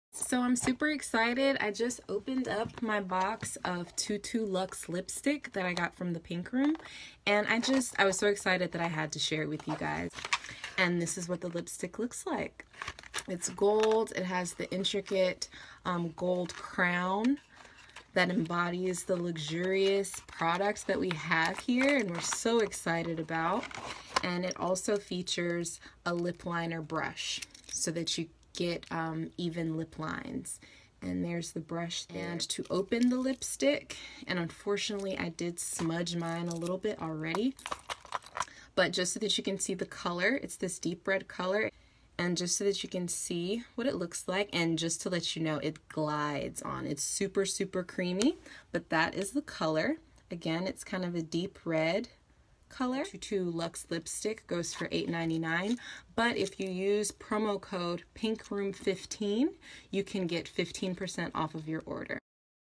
PRODUCT REVIEW: TUTU LUX LIPSTICK
This TUTU lipstick is PERFECT! I purchased it from Aliexpress for less that $5.